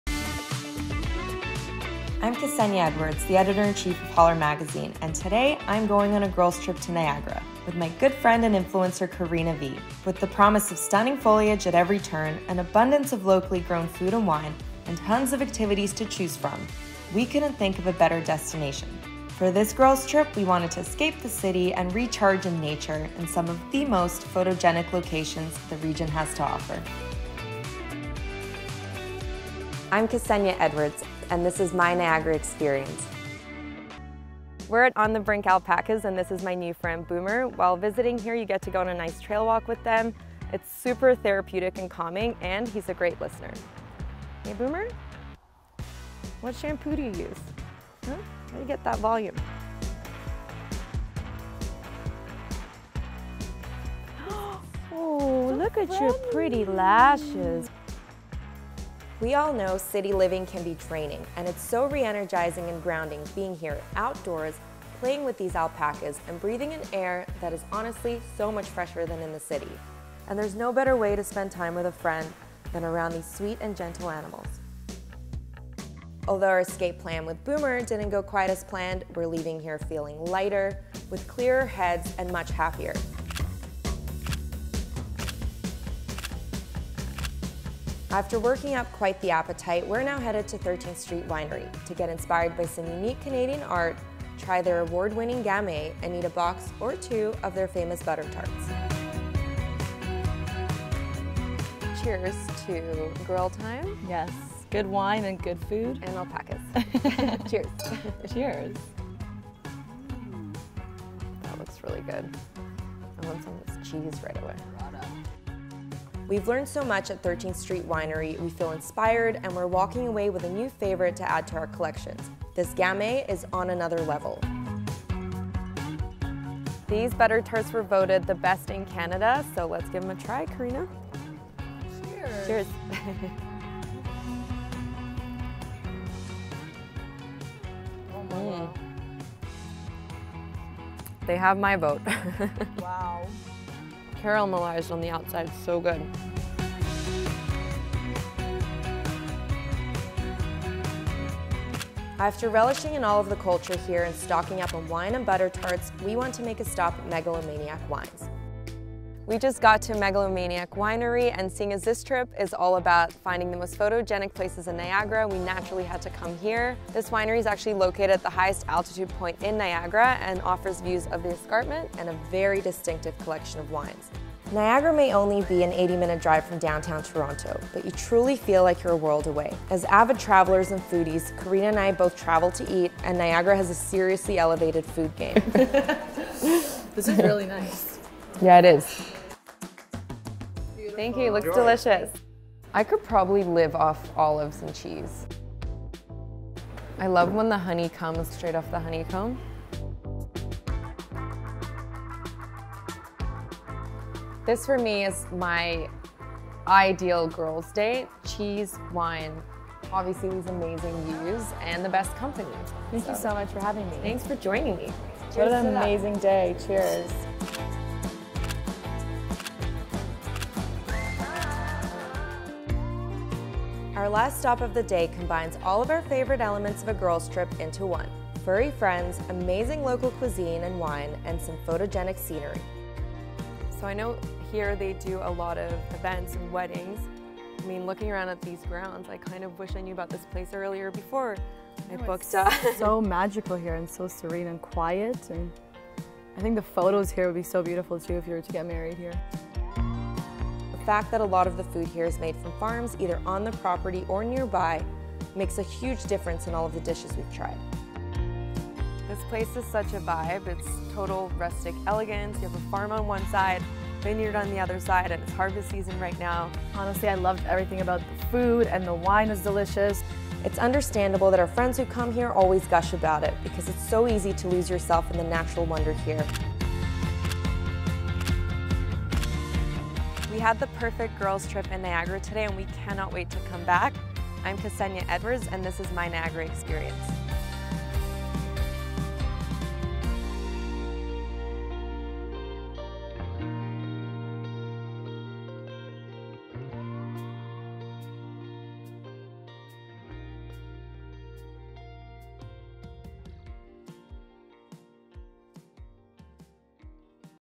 Girls Getaway - Niagara
Girls Getaway: 4 Fun and Photogenic Spots to Visit in Niagara

Escape the city and recharge with these picturesque locations to visit on a girl's trip to Niagara this Fall.

HOLR's Editor-in-Chief Ksenia Edwards and her good friend  Karina Vee @karinalewarne spent a day exploring Niagara, while on the hunt for the most unique and photogenic spots.

There is something so incredibly grounding and re-energizing to get to explore the natural landscapes of a region and Niagara was a breathe of fresh air.

Itinerary:
1. On the Brink Alpacas
2540 Rosedene Rd, Saint Anns, ON L0R 1Y0

What to do: Go on a scenic trail walk with the alpacas. Visit them, feed them and learn all about their quirky personalities inside of their enclosed space
Tip: Visit their website or call ahead to book a

2. 13th Street Winery
1776 Fourth Ave, St. Catharines, ON L2R 6P9

What to do: Explore the unique Canadian art in the 13th Street Art Gallery and the sculpture garden. Eat a box of the best butter tarts in Canada. Learn about their award winning wine , do a tasting and grab a bite to eat.

3. Megalomaniac Wines
3930 Cherry Ave, Vineland, ON L0R 2C0

What to do: Savour one of the best views in Niagara from the restuarant over some wine and a meal, or from the patio. Go for a walk through the vineyards on a wine tour and do a tasting.

4. Honsberger Estate Winery
4060 Jordan Rd, Jordan Station, ON L0R 1S0

What to do: Meet their resident animals on the farm and feed them. Do a wine tasting in the garden and enjoy a locally grown meal. Listen to the live music and walk the ground relishing in the natural beauty and fresh air.

Visiting Niagara is always a great idea, but to do so in the fall and with a close friend, is a truly special and fulfilling experience. If you’re looking to spend some time in Niagara to recharge, uncover unique local cuisine and wine, or just to create some new memories with a friend or loved ones, check out visitniagaracanada.com to learn more.

Video Feature: Ksenia Edwards - Instagram @k.tea.edwards
Karina Vee - Instagram @karinaveee